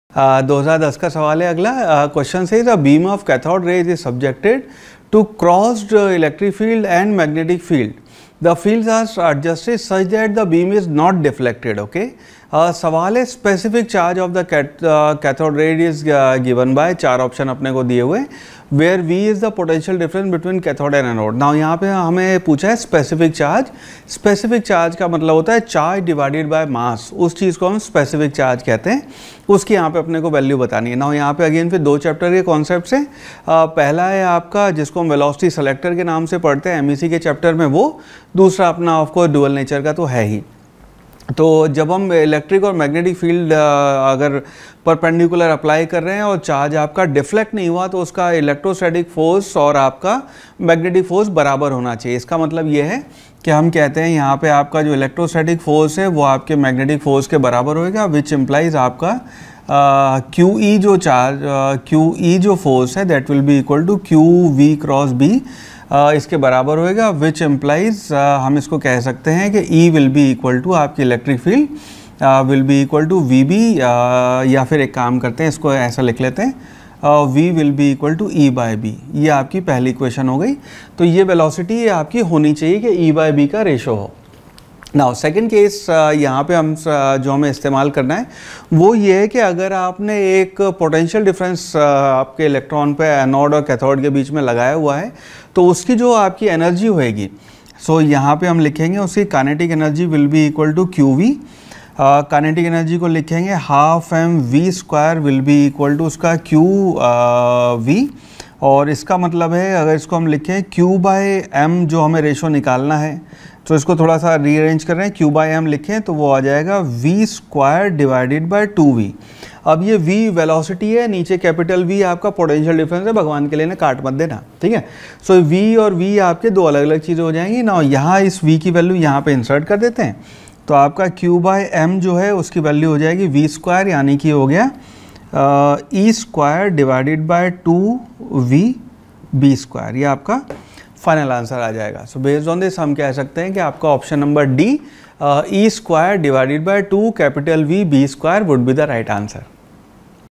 A beam of cathode rays is subjected to crossed electric and magnetic fields. The fields are adjusted
A beam of cathode rays is subjected to crossed electric and magnetic fields. The fields are adjusted such that the beam is not deflected.